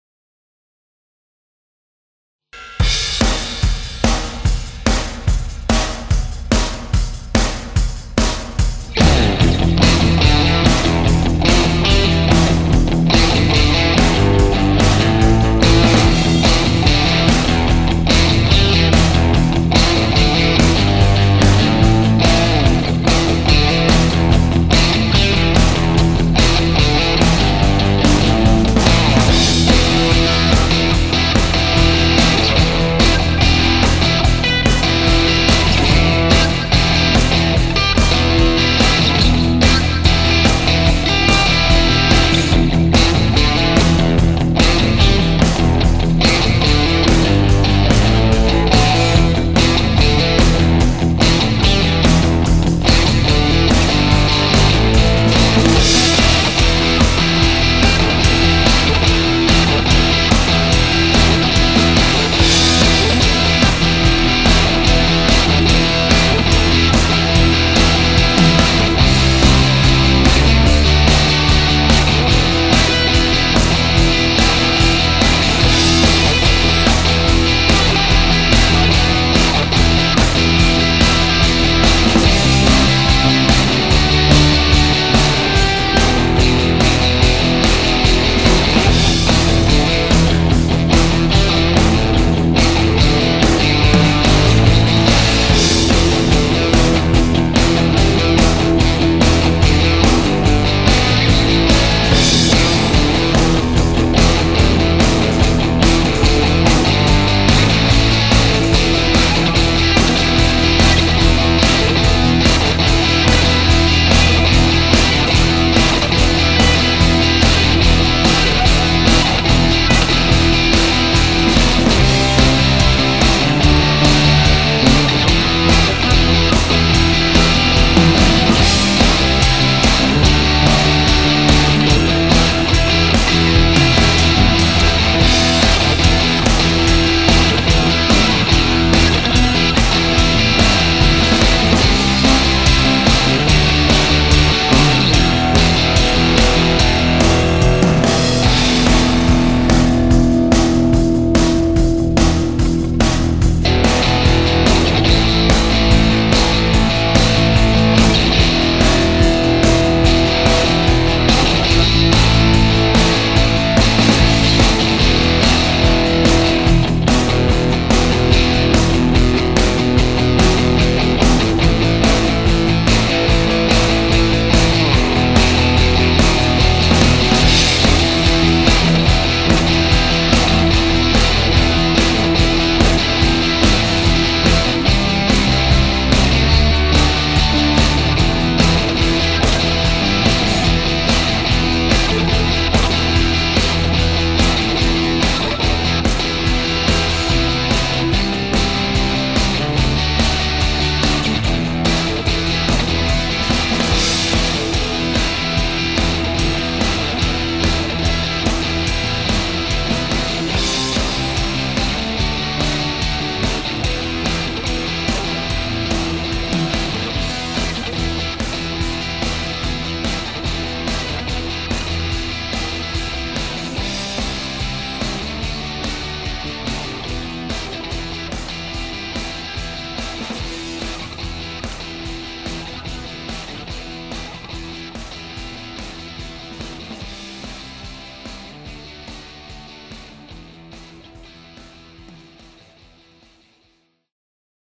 Rock composition
Improvisation of rock music that i made this afternoon , enjoy